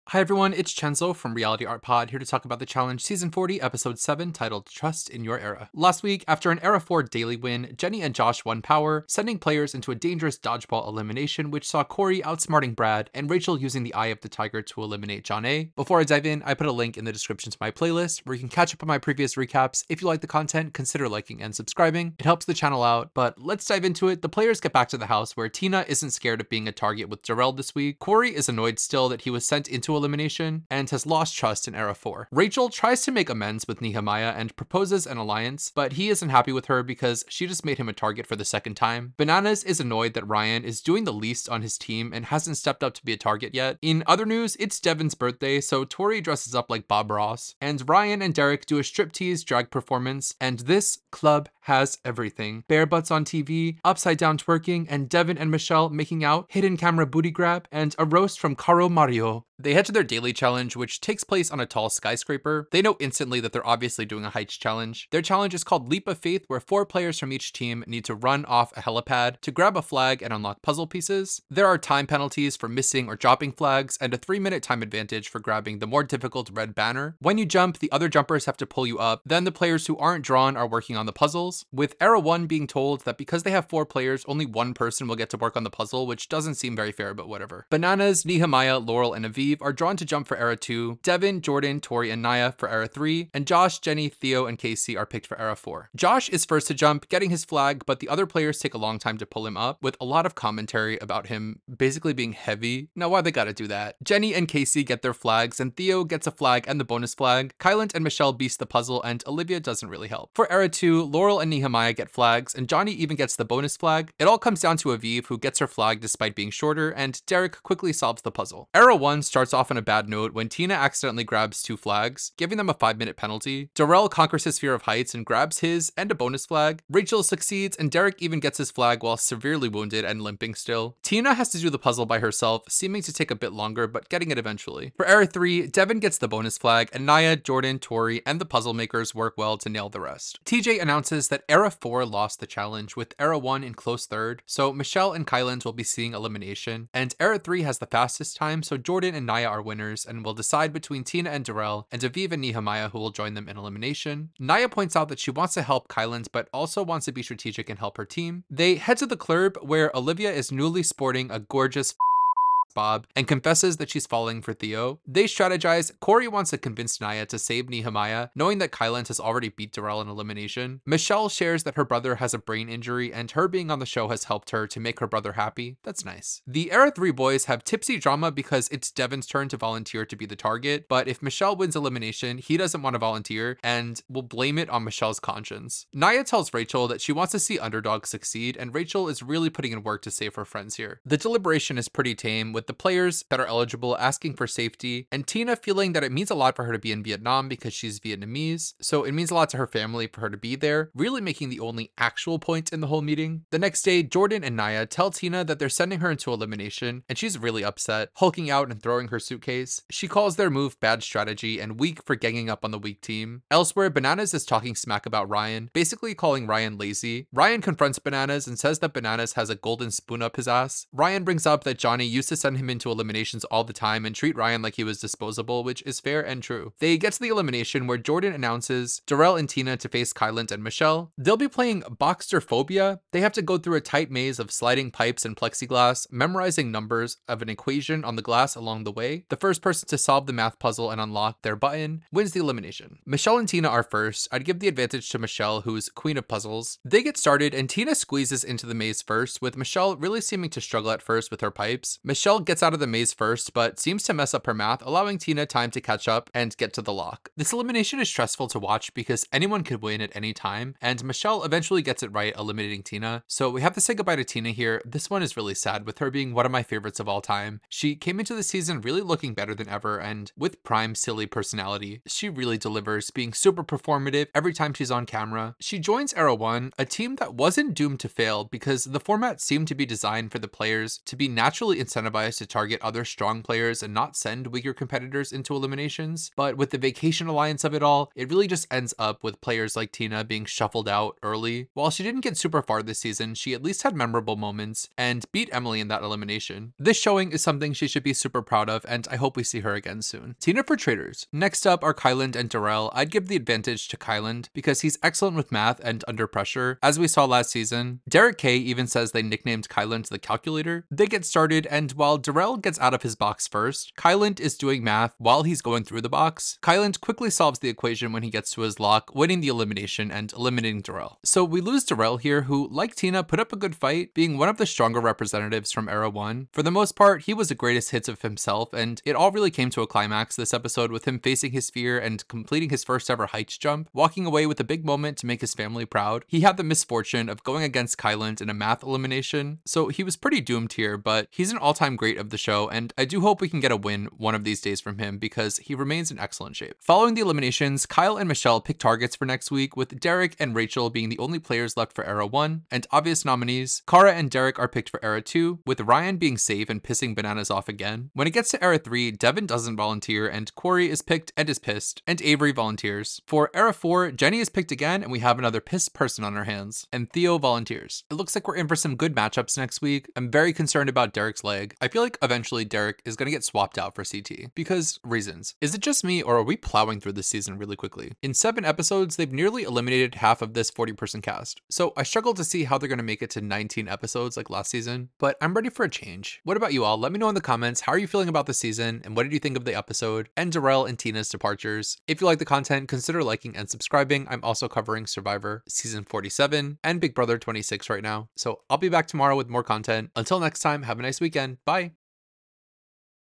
The Challenge 40 - Episode 7 - "Trust In Your Era" Recap & Review - Battle of the Eras
Recapping & Reviewing The Challenge 40 Episode 7 "Trust In Your Era"